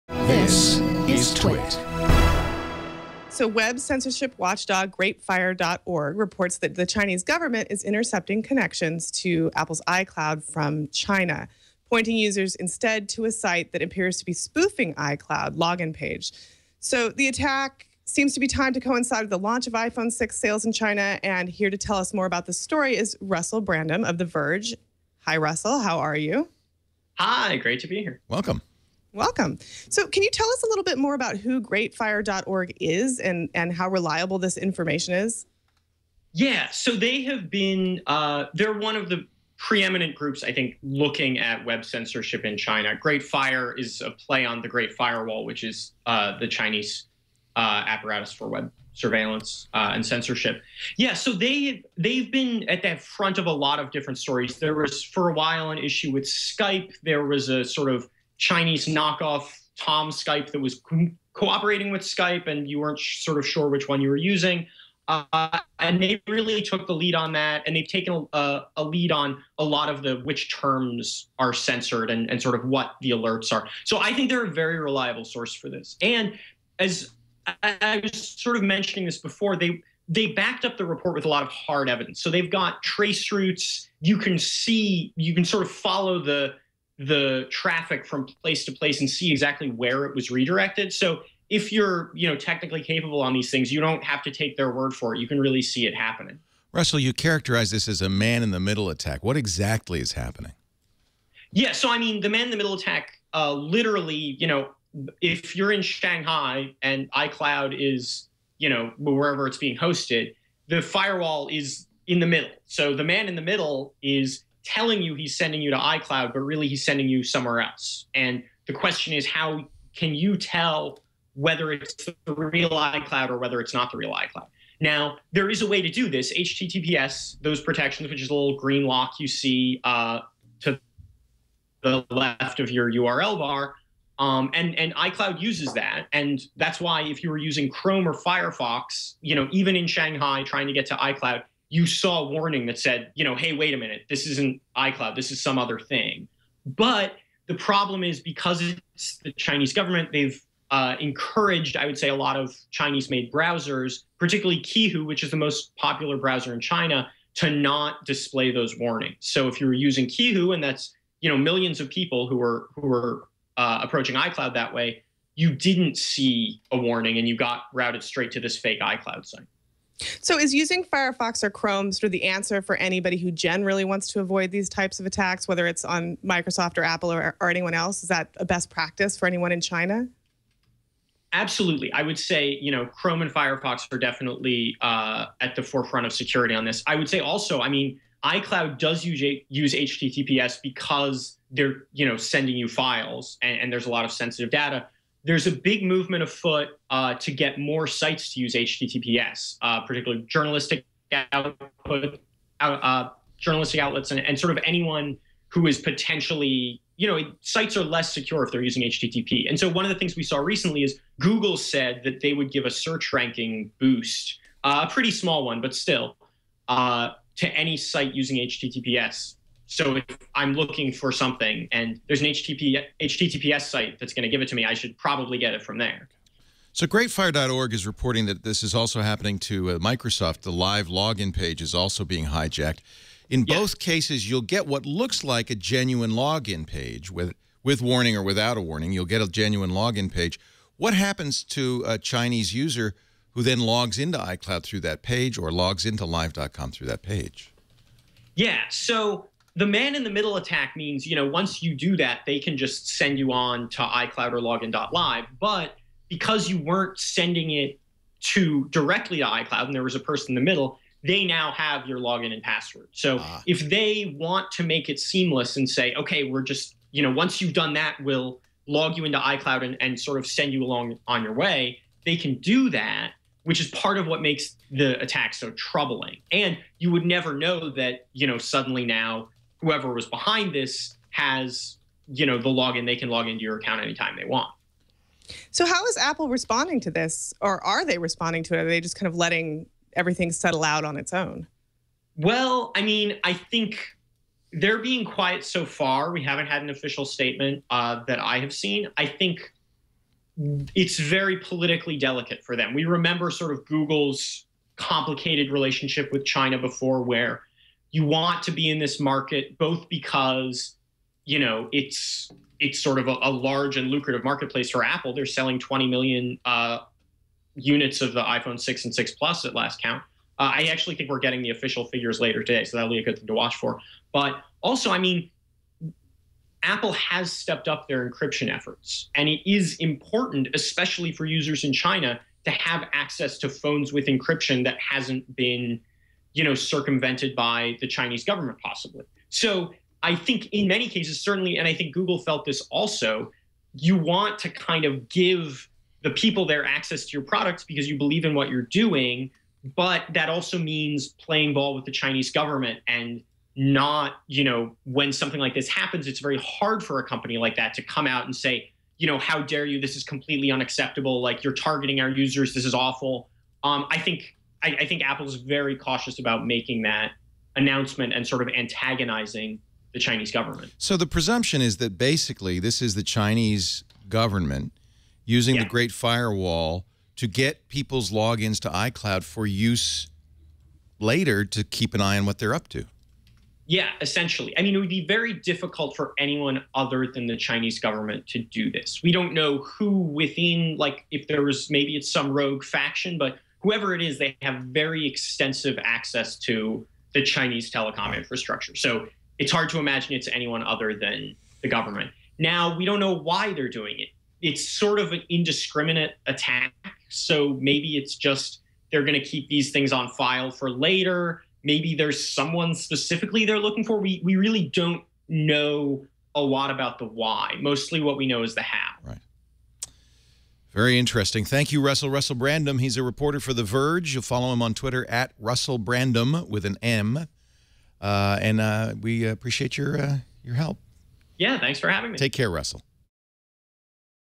China blocks iCloud: Tech News Today 1117
Reports claim that the Chinese government is intercepting connections to Apple's iCloud from China.